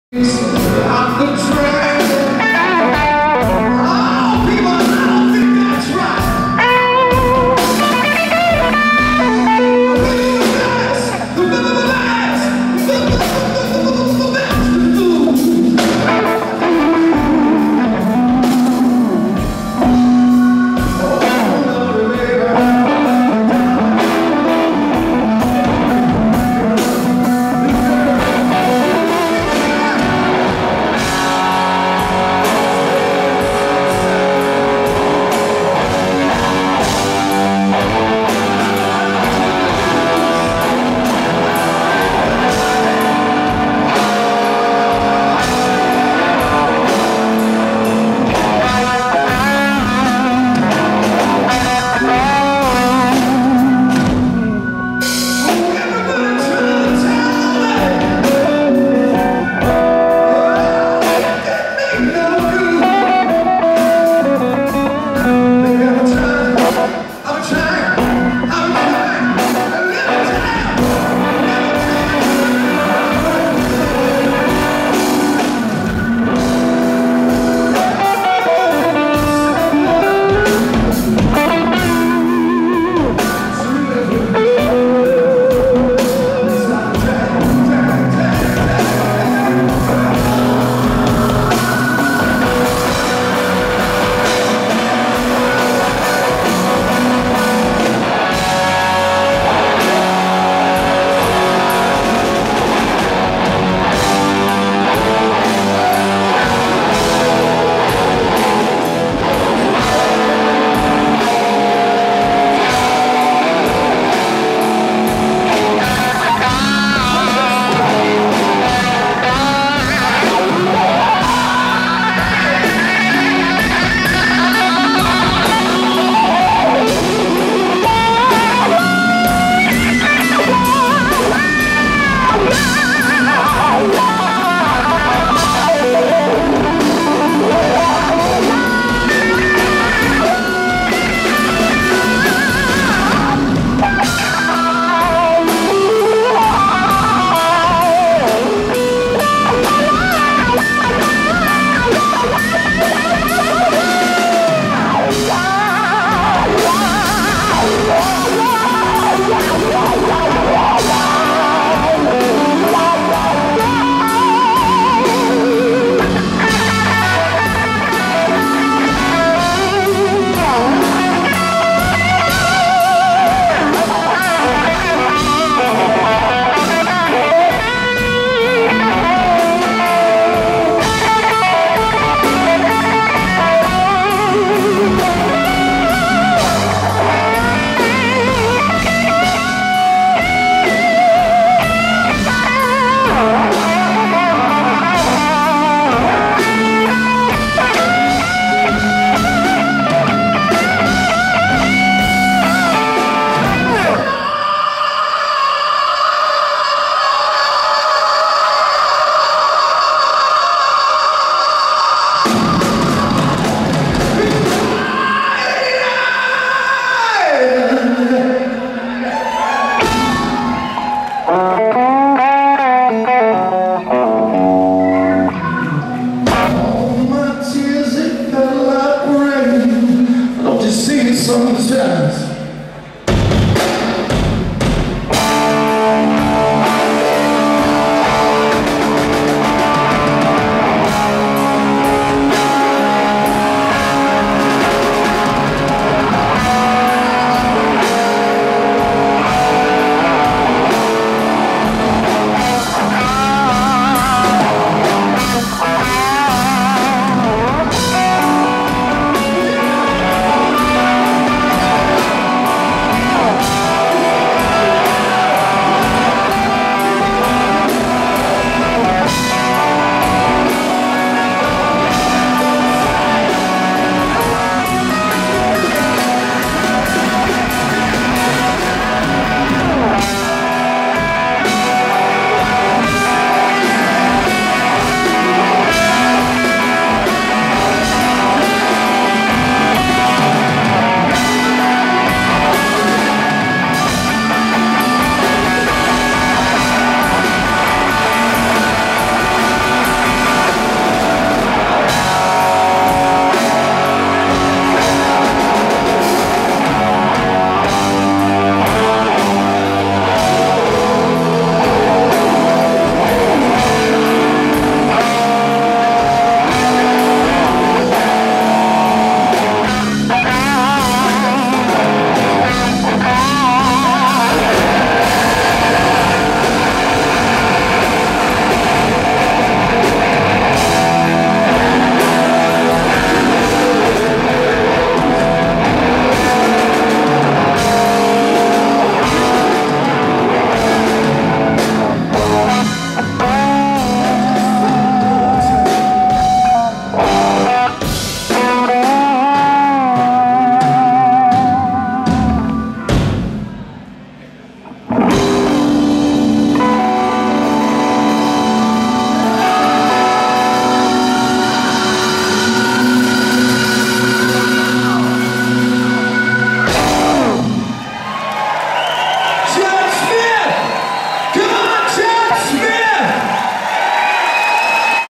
BONZO'16 Chad Smith!!
Chad Smith on drums at Bonzo Bash 2016... worthwhile show, if you missed out, I don't know when we'll get this chance again!! Brent Woods guitar, Michael Devin bass, Stephen Le Blanc keys, awesome song!